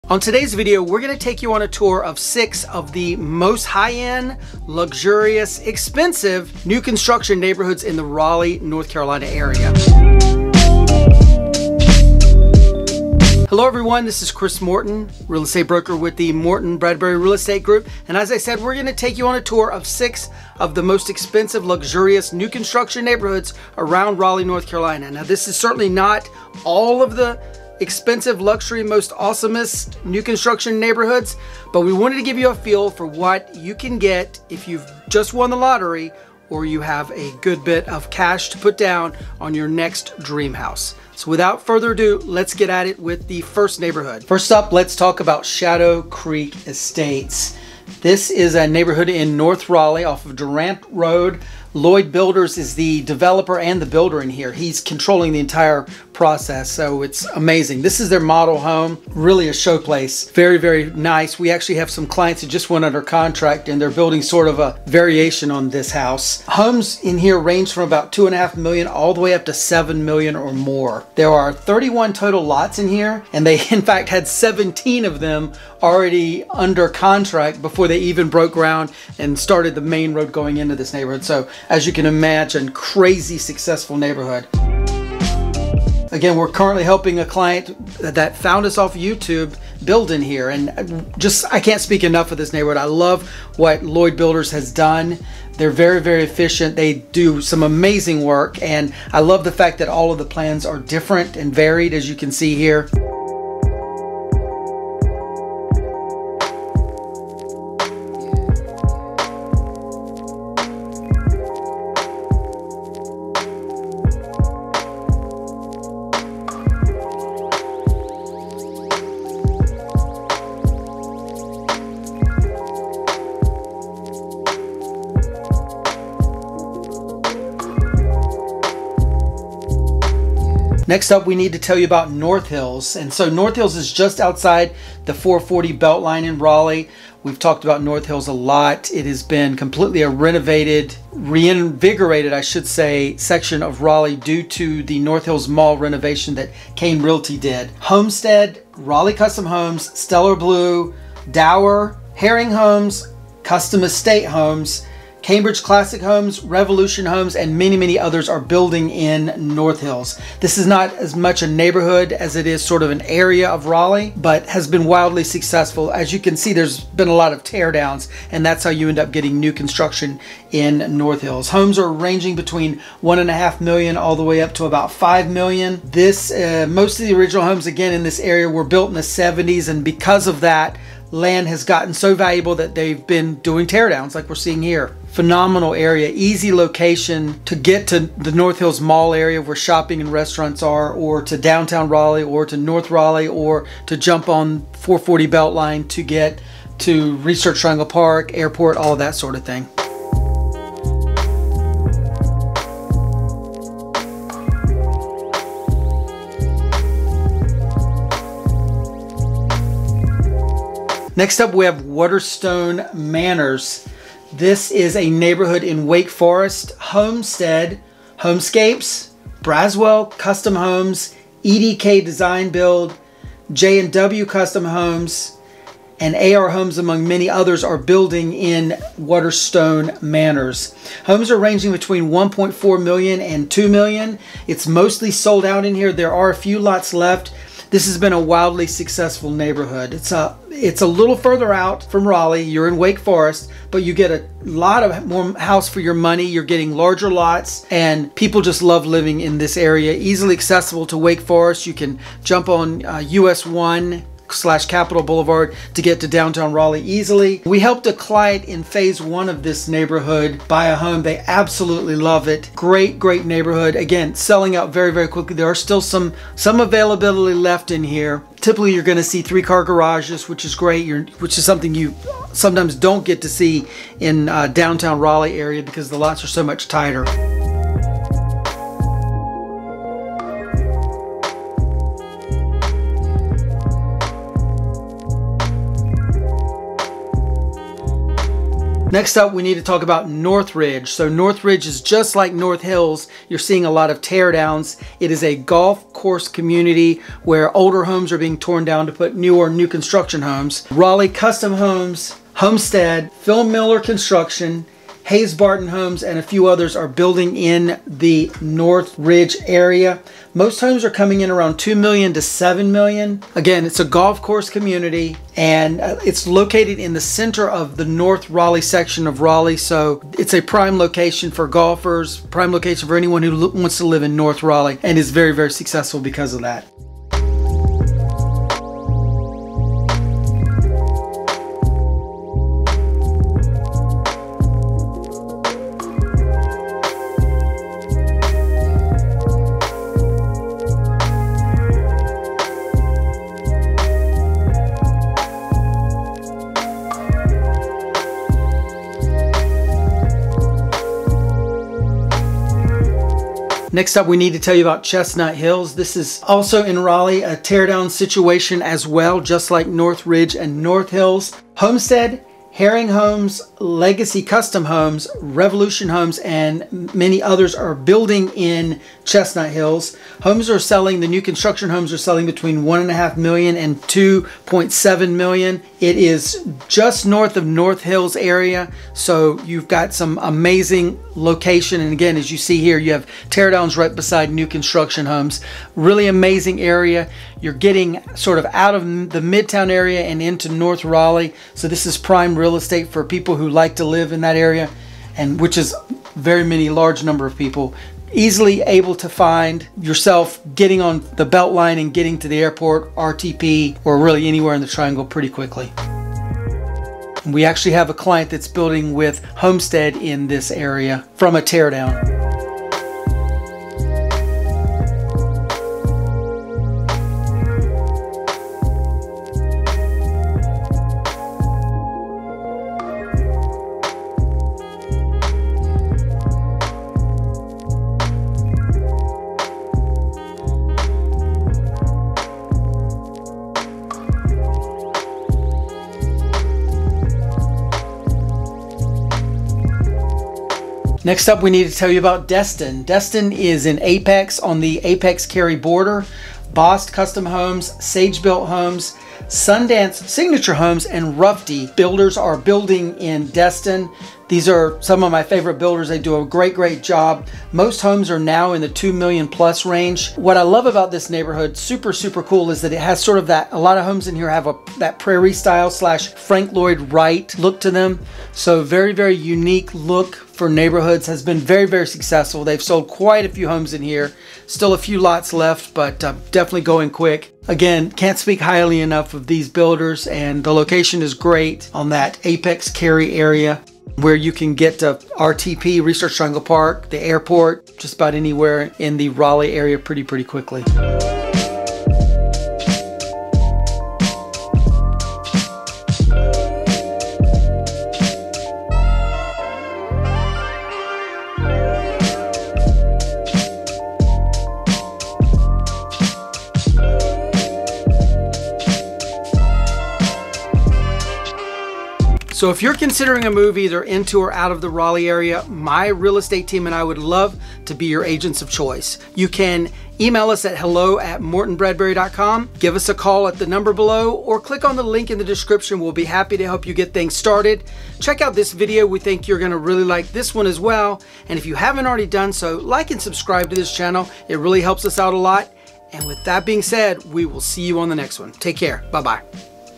TOP 6 Luxury New Construction Neighborhoods in Raleigh NC
We take you on a tour of 6 luxury new construction neighborhoods in Raleigh NC and surrounding suburbs. If you're looking for a luxury new construction home, want to see what Raleigh NC builders are up to, or just want to see some of the most expensive neighborhoods in the Raleigh North Carolina area, you definitely need to watch this video! We show you Shadow Creek Estates, North Ridge, Destin, North Hills, Chestnut Hills, and Waterstone Reserve.

🏠 If you're thinking about buying or selling a home in NC, please click here to take our short questionnaire!

New video out every Tuesday and Friday!